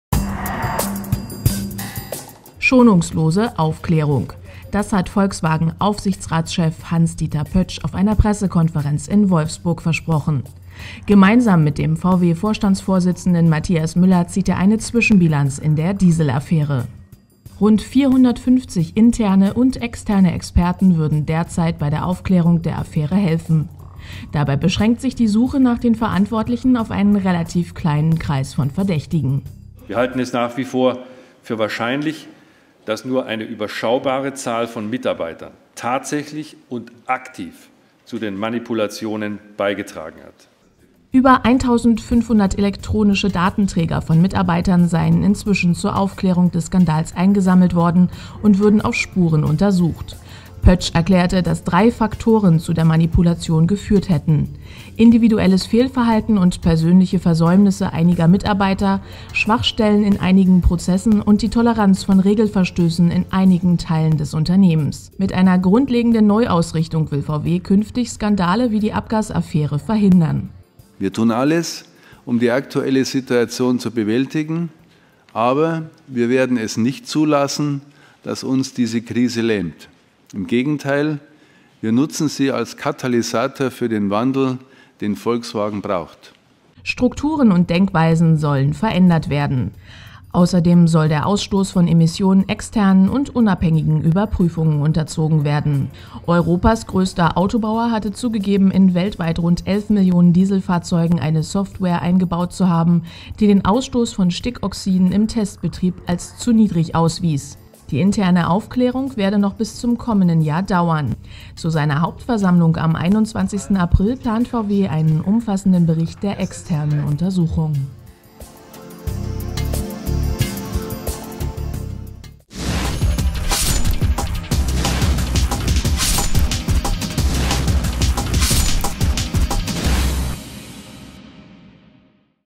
VW-Konzern kommt bei seiner Neuausrichtung gut voran
Der Volkswagen Konzern kommt bei seiner Neuausrichtung gut voran. Bei allen fünf Ende Oktober vorgestellten Prioritäten macht der Konzern Fortschritte: Die technischen Lösungen für die Kunden in Europa sind erarbeitet, den Behörden vorgestellt und dort positiv bewertet w